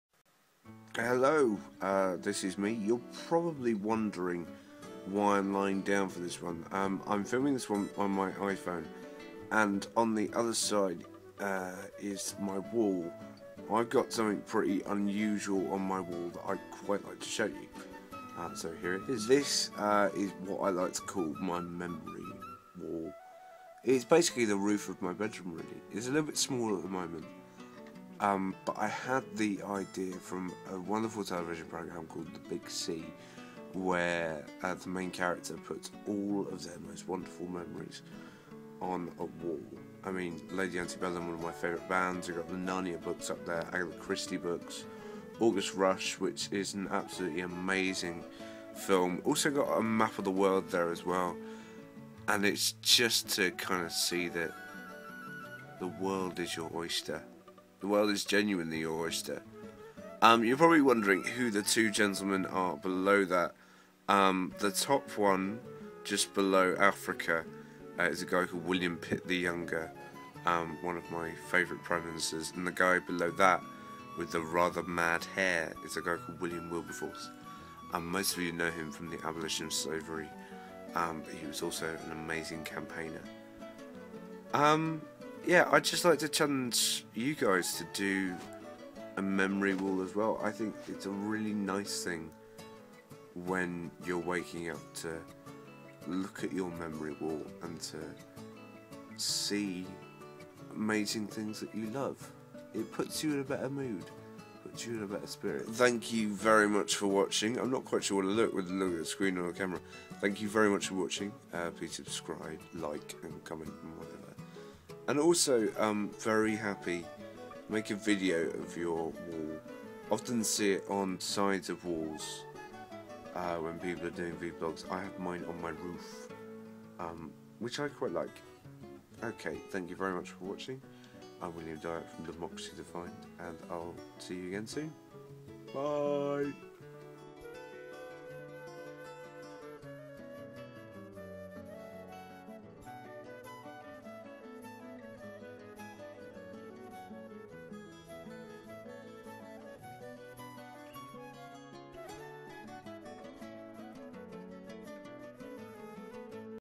Vblog: MY MEMORY WALL TAG
My Memory Wall is a very special part of my life and one that I want to share with you. I hope you enjoy.